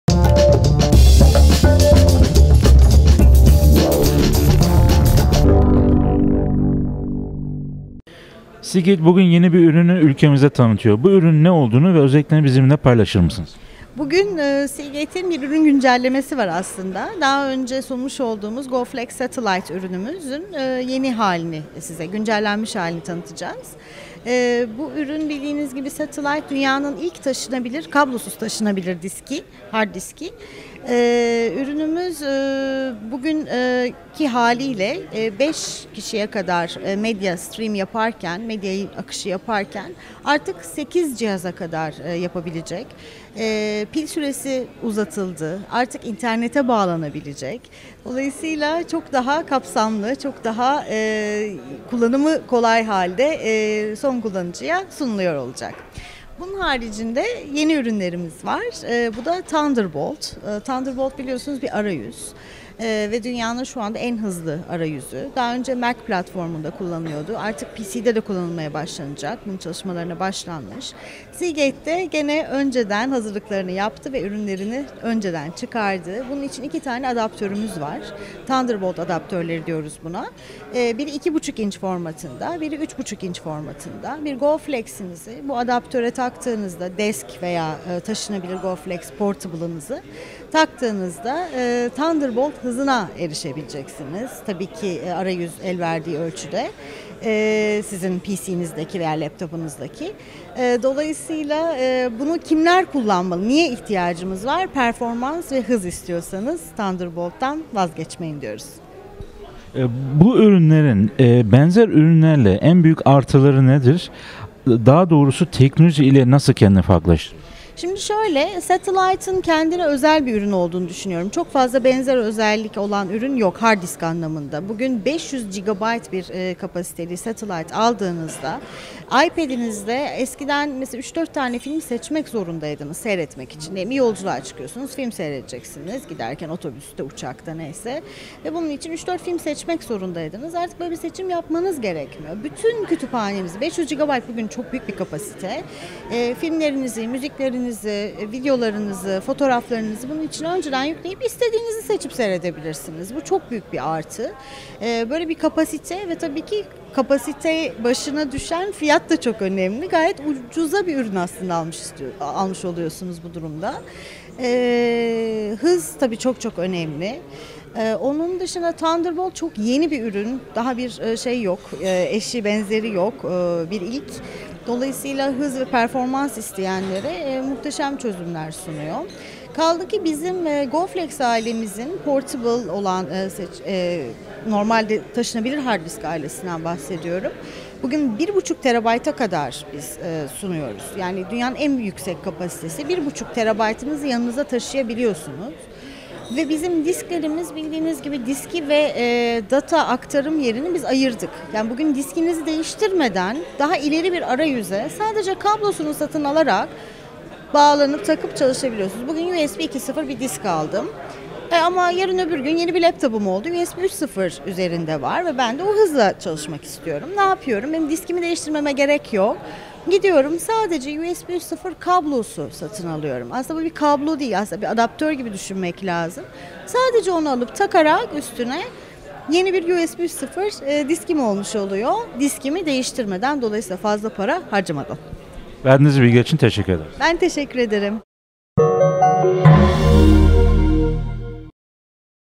Seagate GoFlex Satellite ailesini güncelledi
Seagate yapmış olduğu güncelleme ile GoFlex Satellite ailesine yeni özellikler kattı. Detaylar videomuzda.